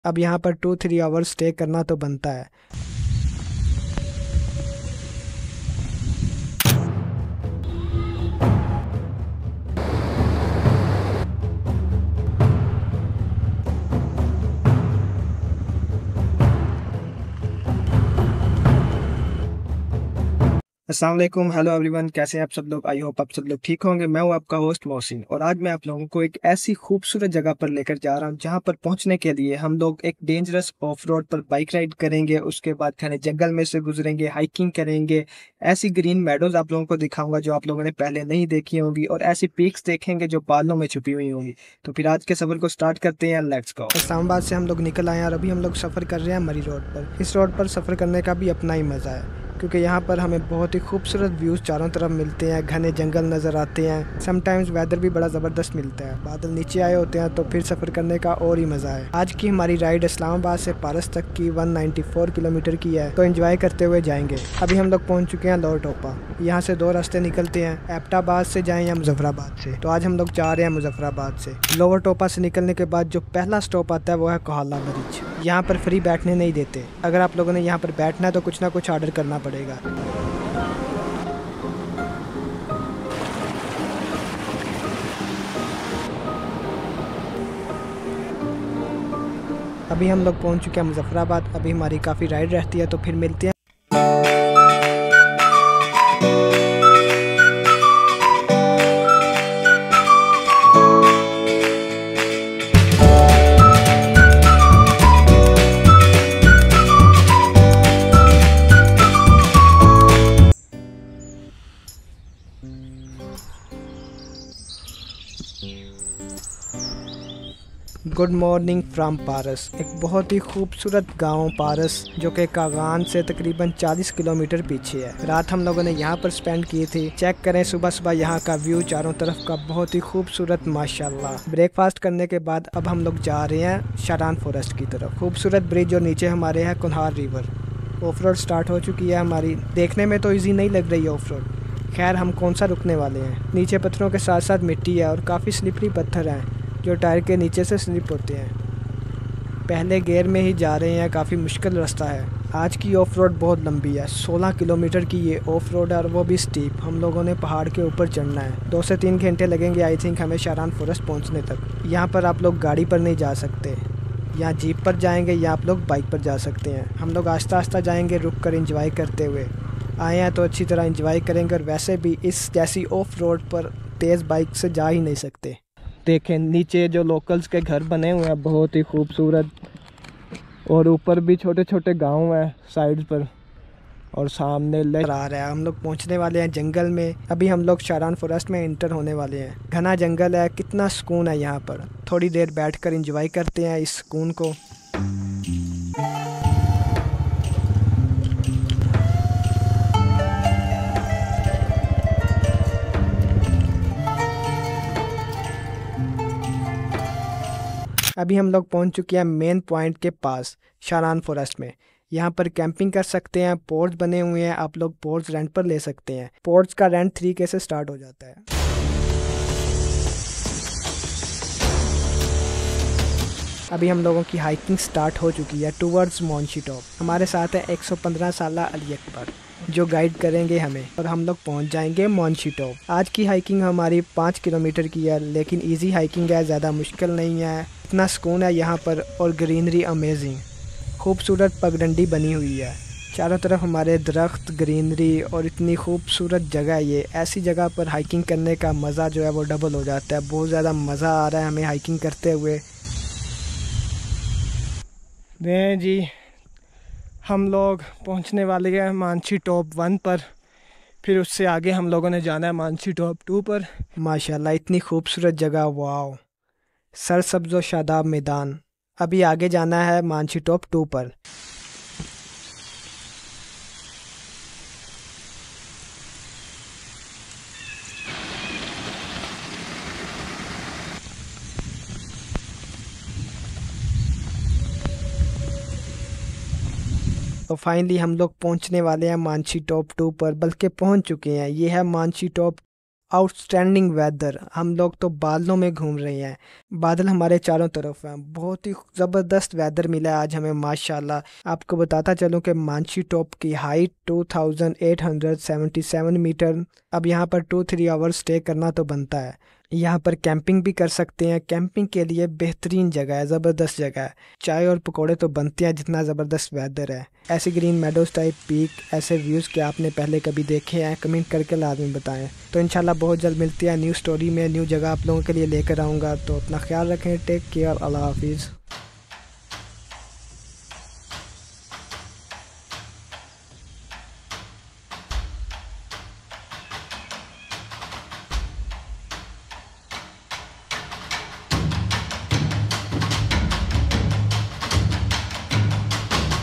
What's Hiding in the Shadows of Wild SHARAN Forest's Manshi Top? Ep.1
Join me on an epic adventure as I explore the breathtaking Sharan Forest in Pakistan! From tackling its challenging off-road trails to hiking through dense, untouched jungles, this journey is packed with excitement. The final destination? The stunning Manshi Top, where green meadows and panoramic mountain views await. Get ready for an adrenaline-filled ride through nature's finest landscapes, with a mix of thrilling jungle treks, serene meadows, and unforgettable moments. This is one adventure you don’t want to miss!

I use music in this video from these amazing websites

Sound Effect by Gregor Quendel from Pixabay
Sound Effect by Soul_Serenity_Ambience from Pixabay